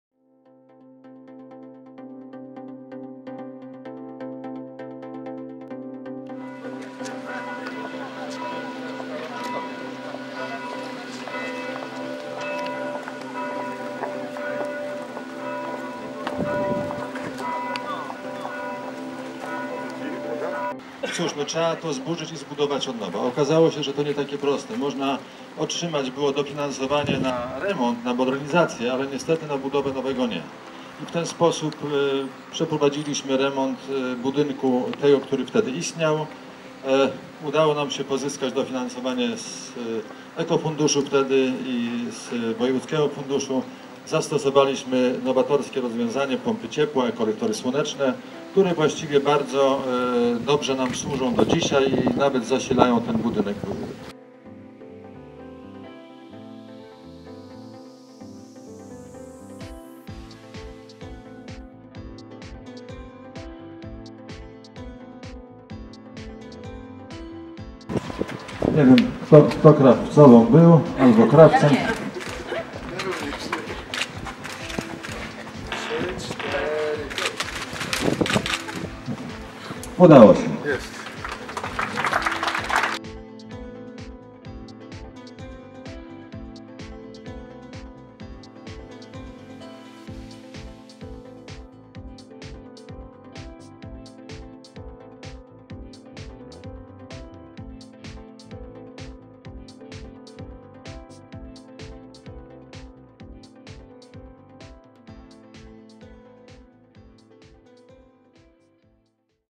Puck: Otwarcie Domu Żeglarza przy HOM w Pucku
28 września 2017 r. odbyło się uroczyste otwarcie "Domu Żeglarza".